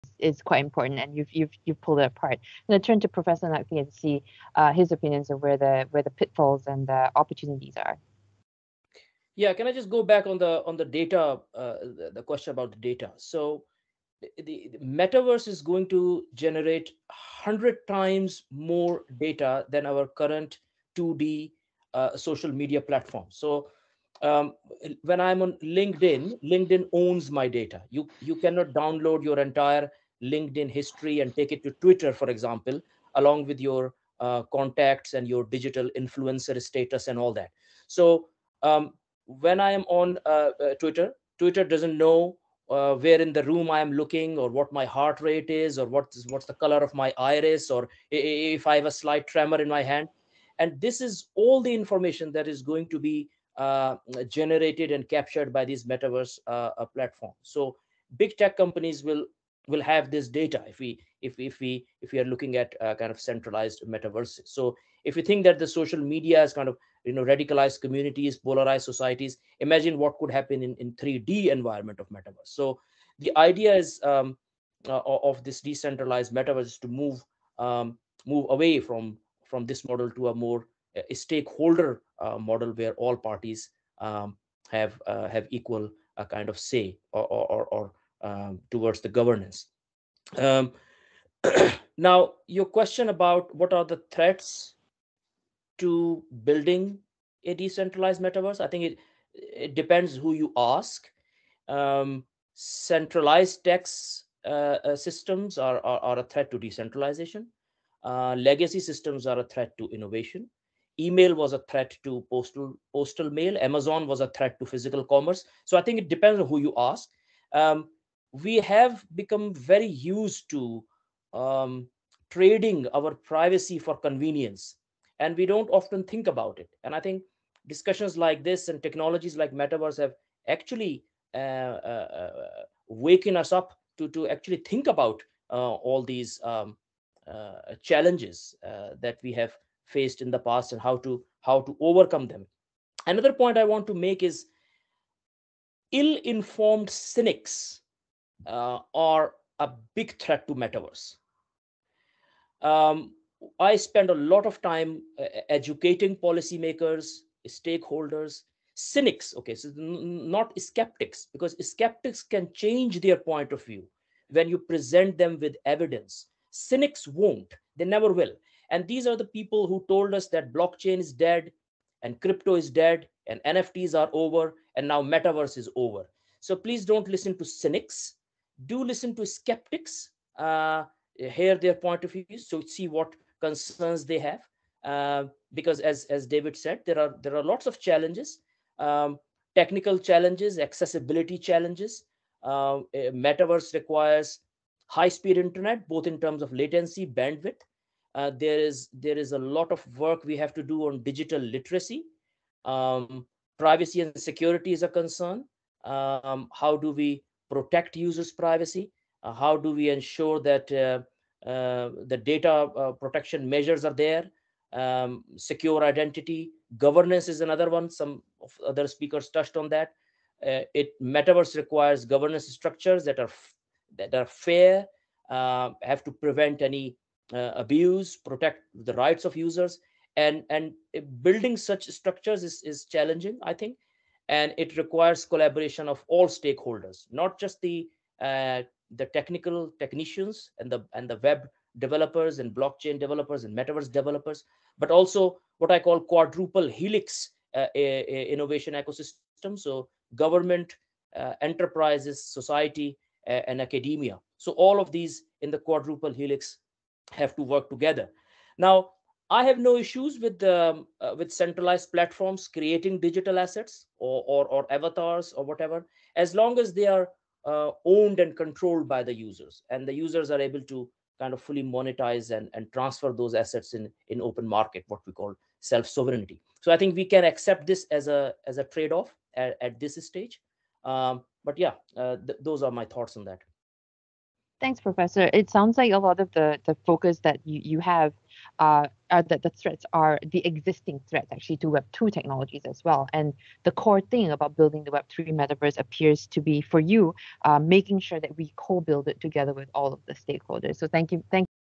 What are the Major Threats to Building an Open Decentralised Metaverse?
BBA President, Prof. Dr Naseem Naqvi MBE shares his thoughts at Access Partnership's Forum.